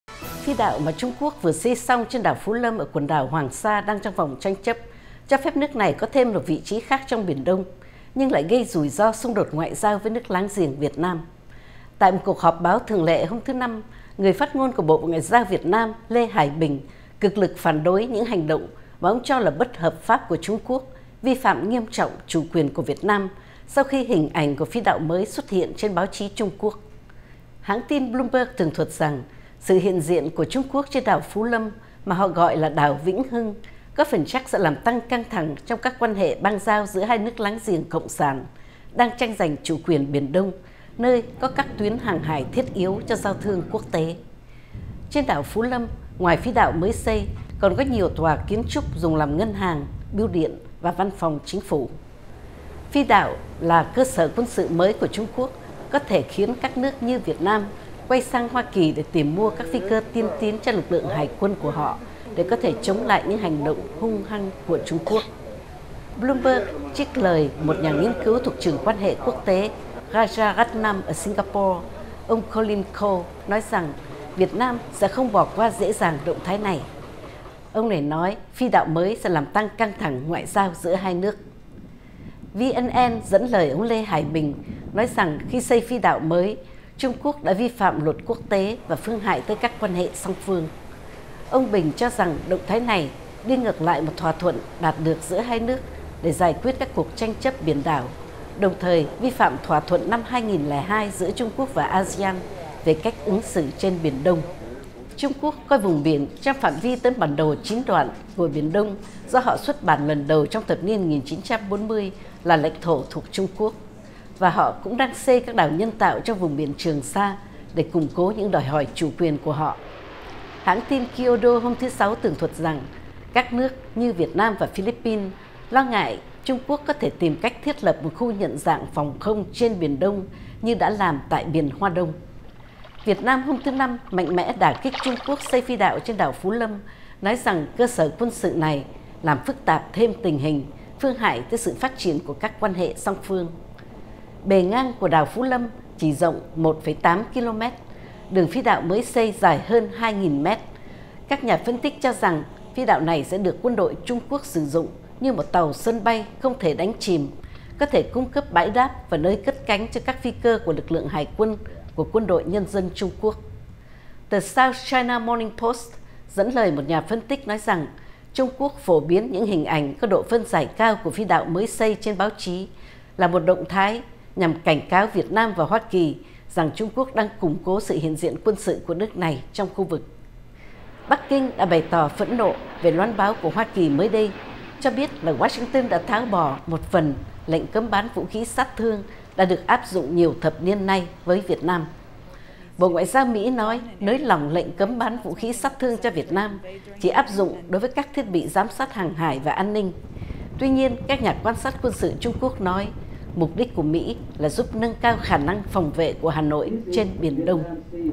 Phi đạo TQ xây trên đảo Phú Lâm ở Hoàng Sa gặp phản ứng mạnh của VN
Tin tức Nếu không vào được VOA, xin các bạn hãy vào để vượt tường lửa. Phi đạo mà Trung Quốc vừa hoàn tất trên đảo Phú Lâm ở quần đảo Hoàng Sa đang trong vòng tranh chấp, cho phép đem lại thêm một thế mạnh cho nước này ở Biển Đông, và có nguy cơ châm ngòi cho một sự rạn nứt nữa về ngoại giao với nước láng giềng Việt Nam. Tại một cuộc họp báo thường lệ hôm thứ Năm, Người phát ngôn của Bộ Ngoại giao Việt Nam Lê Hải Bình gọi phi đạo dài 2 kilomet này là một hành động vi phạm nghiêm trọng chủ quyền của Việt Nam, sau khi hình ảnh về dự án này xuất hiện trên báo chí Trung Quốc hồi trong tuần.